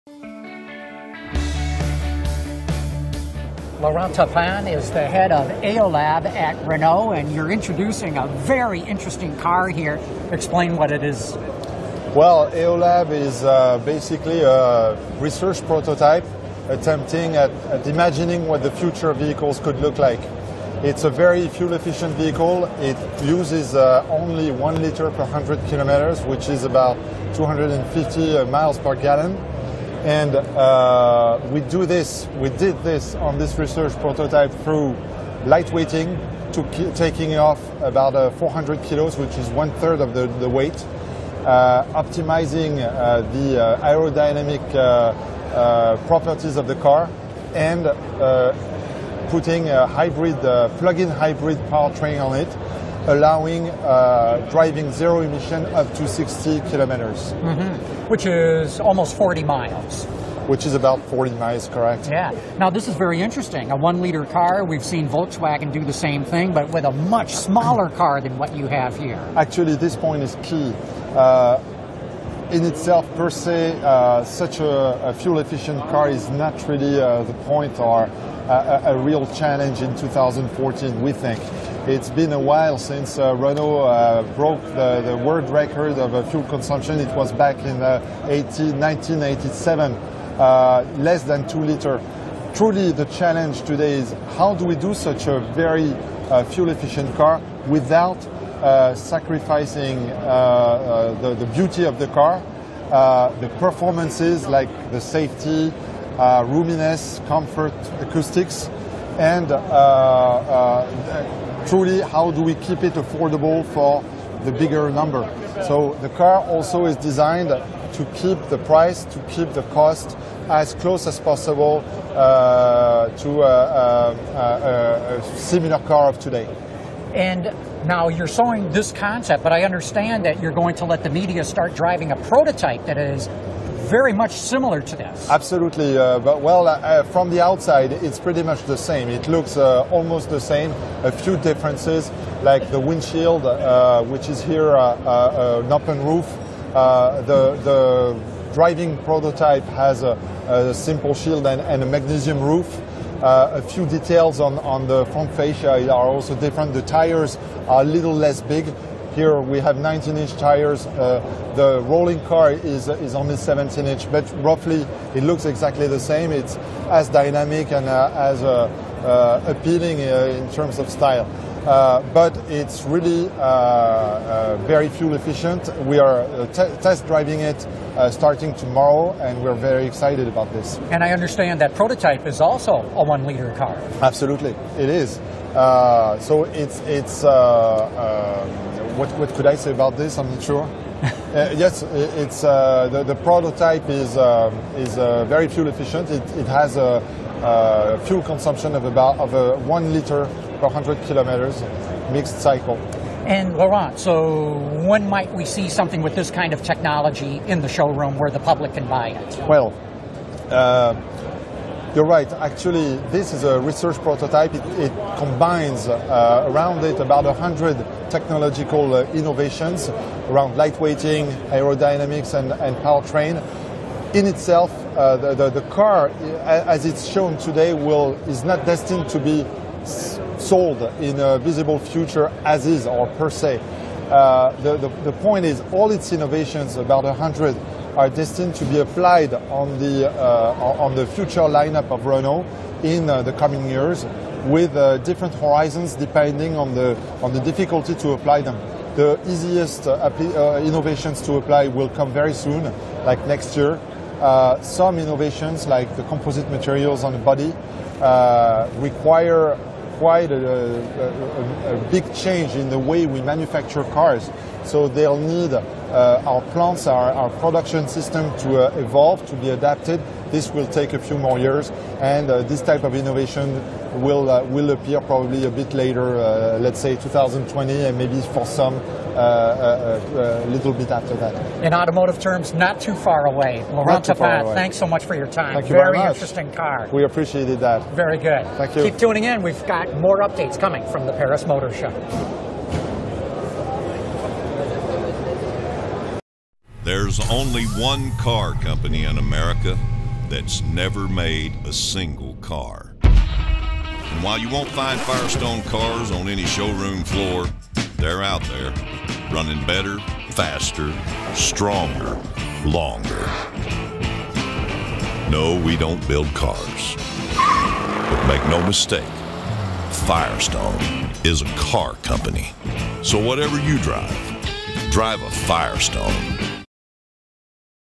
Renault 1L Eolab Bests VW in Function, Style - Paris Motor Show 2014
The Renault Eolab may be a concept on the Paris Motor Show floor, but drivable prototypes are around the corner. Laurent Taupin, Project Chief of the Eolab, says the prototypes will even carry through most of the flair of the model on the turntable, all while getting a head-turning 250 MPG. How did they do it, and what is the ultimate fate of this one-liter wonder?